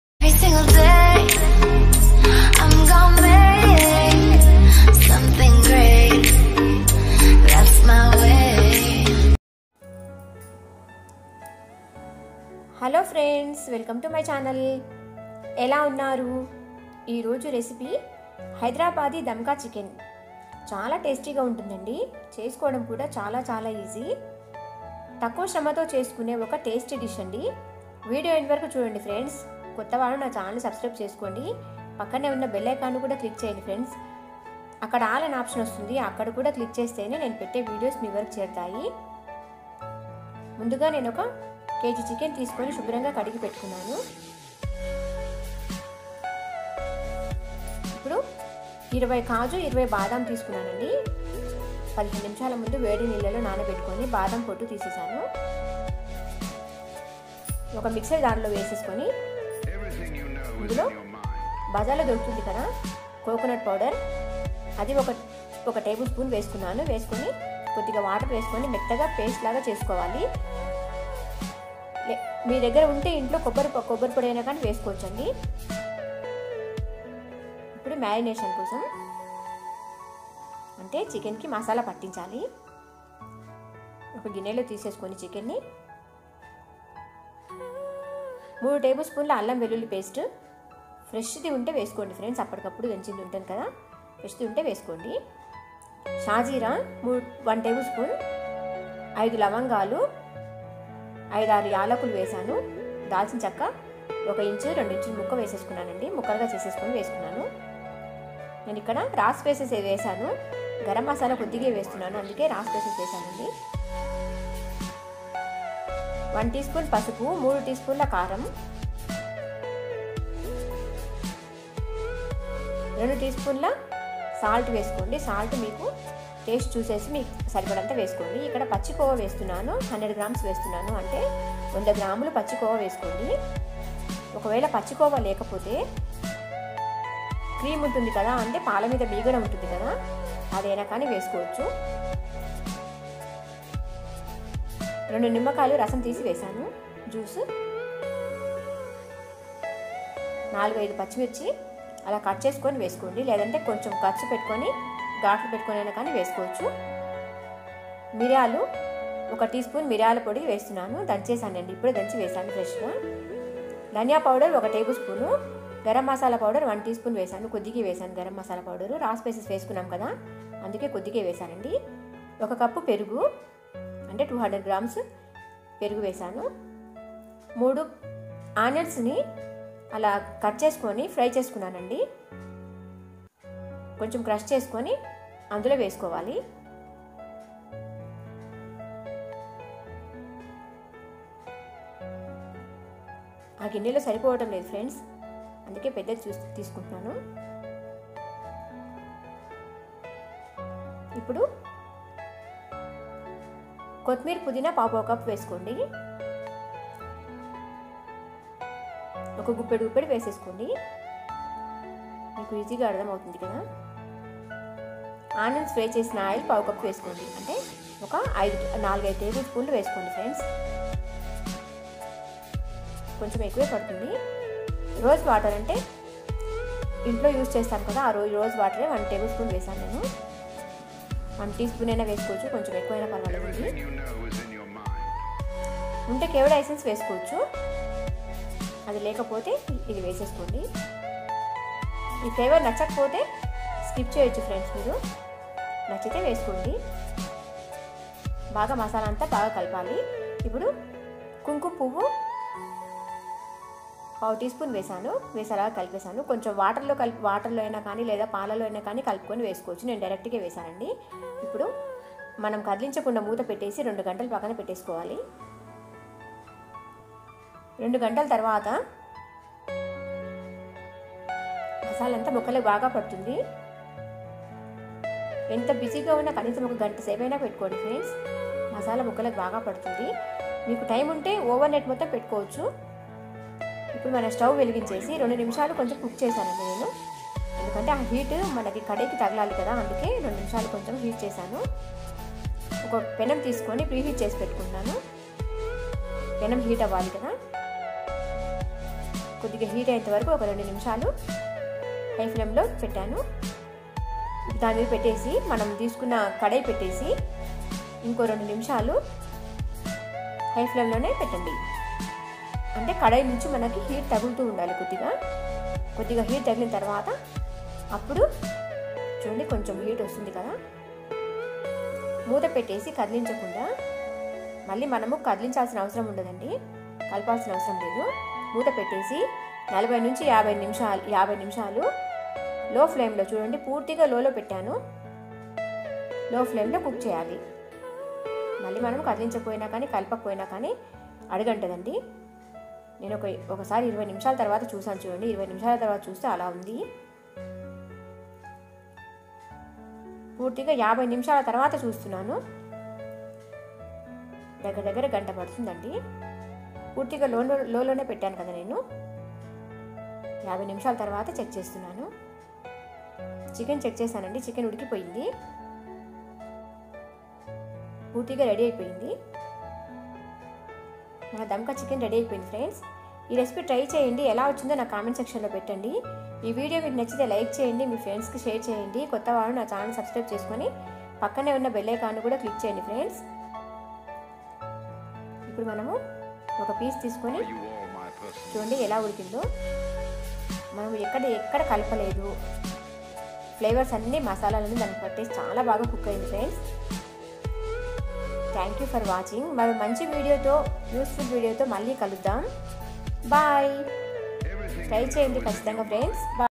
Hyderabadi Dum Ka Chicken || హైదరాబాది దమ్ కా చికెన్ ఒక్క సారి ఇలా చేసి చూడండి ఆహా .. 😋👌అంటారు
హైదరాబాది దమ్ కా చికెన్ ఒక్క సారి ఇలా చేసి చూడండి ఆహా...😋👌 అంటారు .

Ingredients :

Almonds - 20
ginger garlic paste - 3 tbsp
Shahjeera -1 tbsp
Cloves - 5
Cardamom - 6
Khova/Maava - 100 gm
2 lemons juice
Curd - 1 cup (250gm)
Friend onions - 3
Oil - 1/4 cup (4-5 tbsp)
Rose water - 1-3 tsp
Zafran - 1/4 tsp